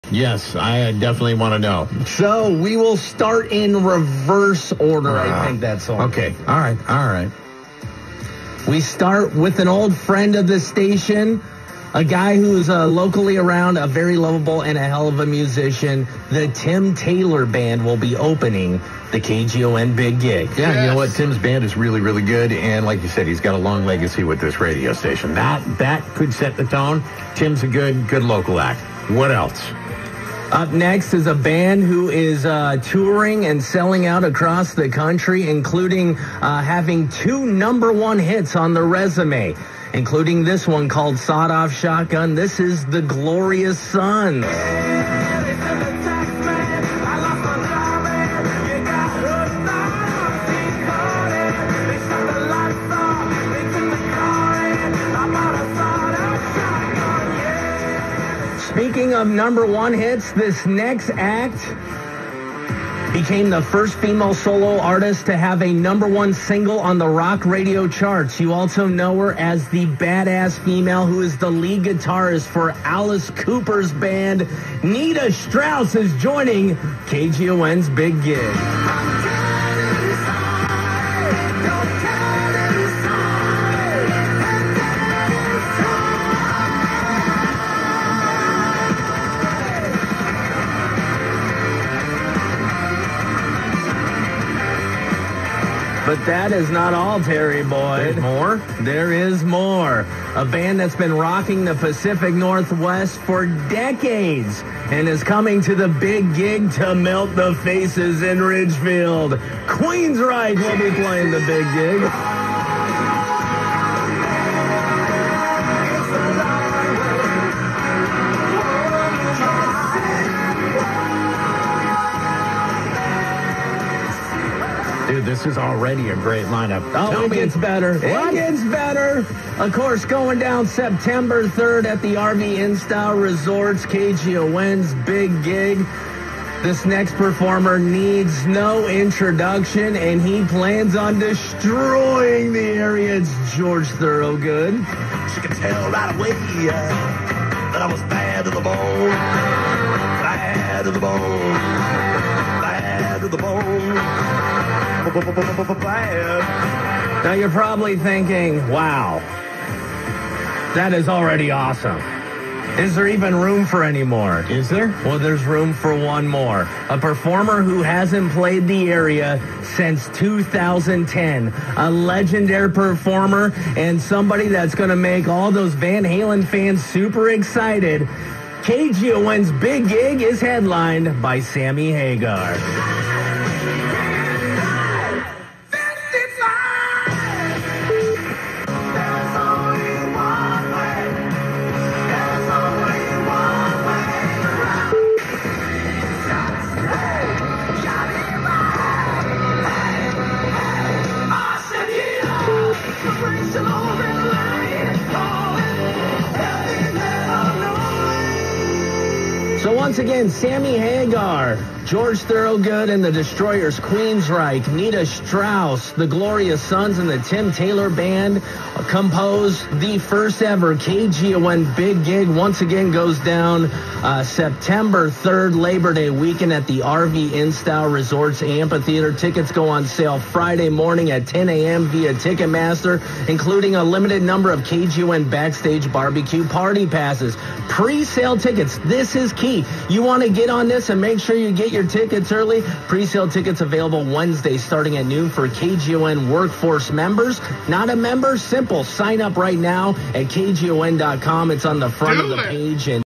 The Big Gig announce on KGON!
The show is Saturday September 3rd at RV Inn Style Resorts Amphitheater in Ridgefield, WA.